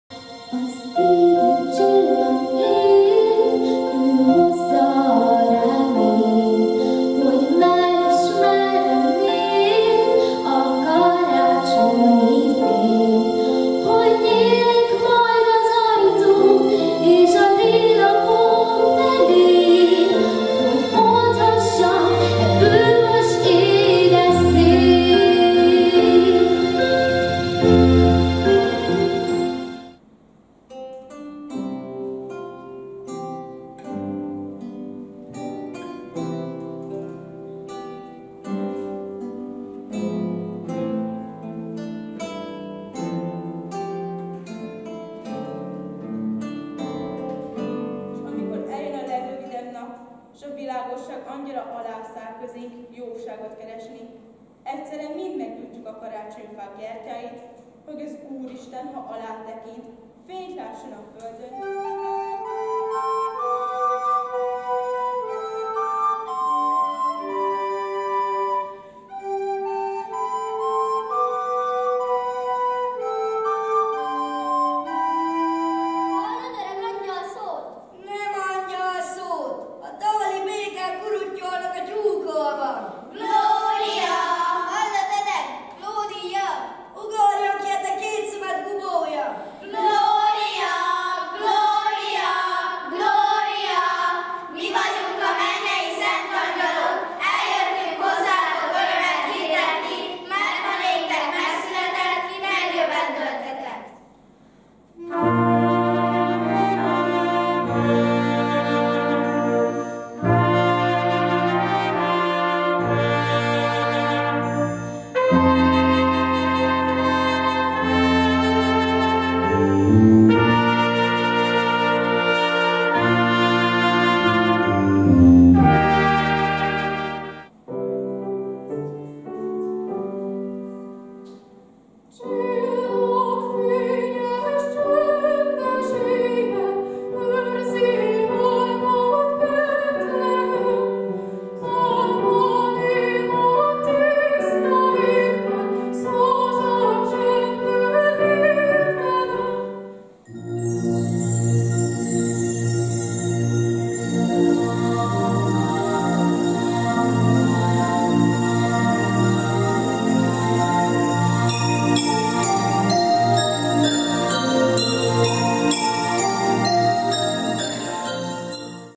Karácsonyváró mini műsor 2. nap ízelítő
Egy kis ízelítő a Kazincbarcikai Kodály Zoltán Alapfokú Művészeti Iskola karácsonyváró koncertsorozatának második napjáról.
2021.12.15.